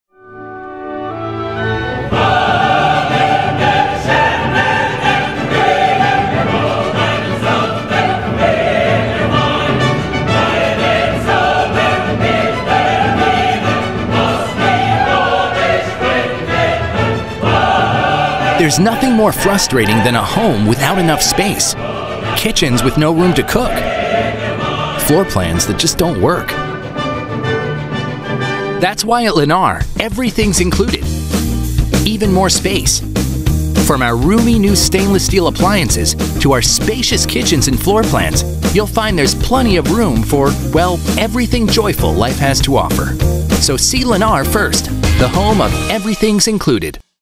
With Lennar, Everything's Included, Even Space!
Are you stuck in a kitchen with no room to cook or in a floor plan that just doesn't work? We can help!